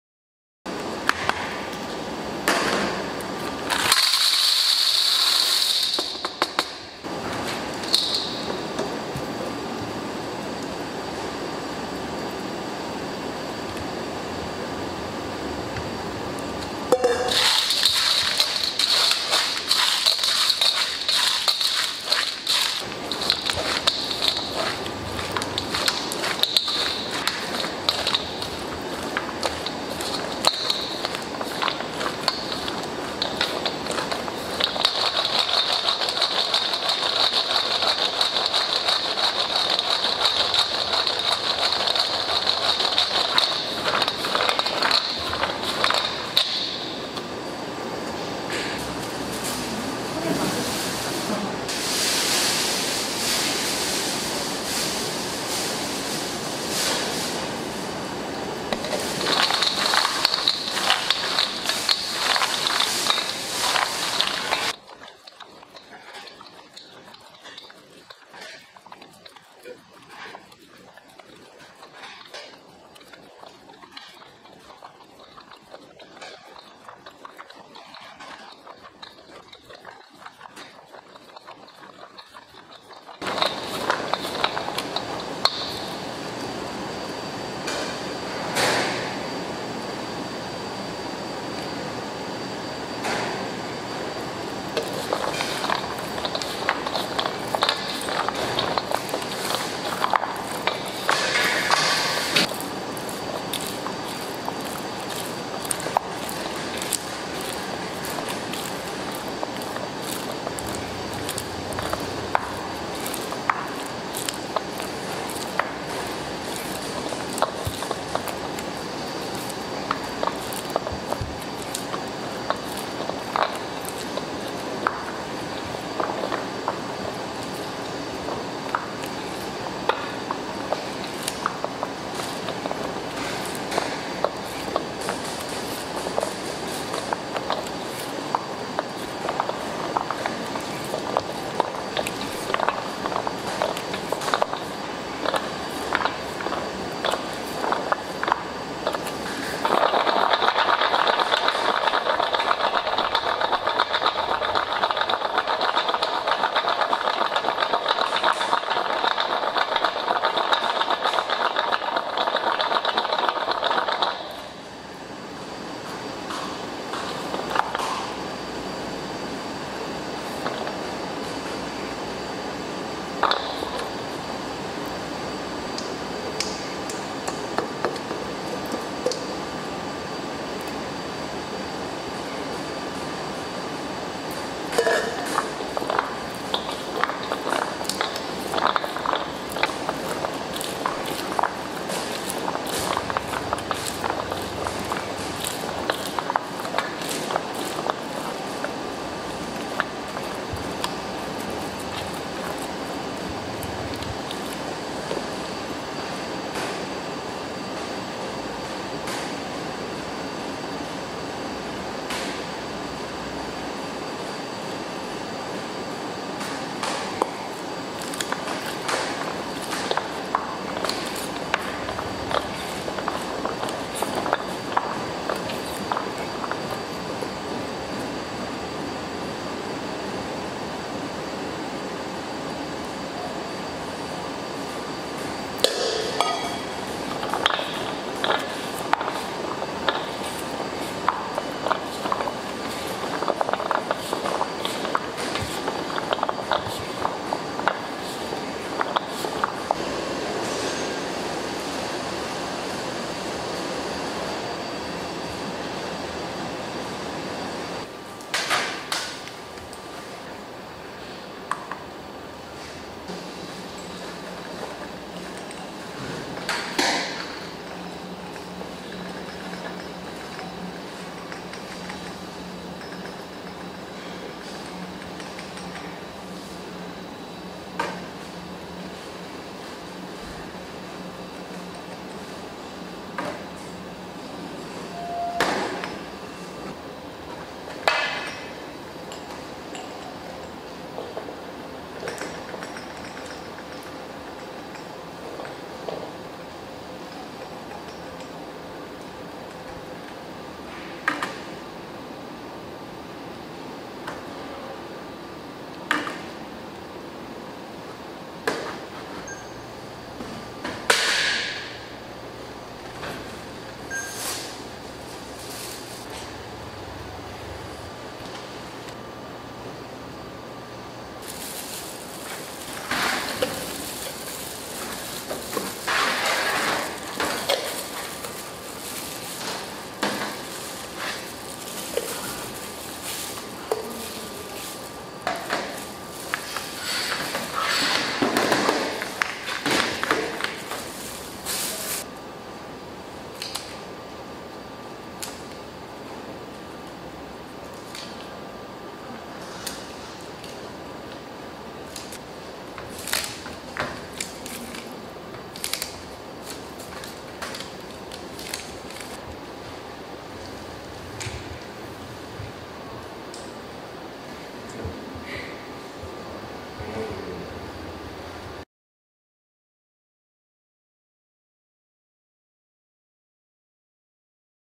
2시간동안 한겹한겹 다크초콜릿을 입히는 과정으로 탄생한 아망드 쇼콜라 수제 초콜릿 / Almond chocolate / Korean Chocolate
💖 김보람 초콜릿 💖

💚 아망드 쇼콜라 : 캬라멜을 입힌 볶은 아몬드에 다크초콜릿을 겹겹이 입히고 카카오파우더로 마무리한 초콜렛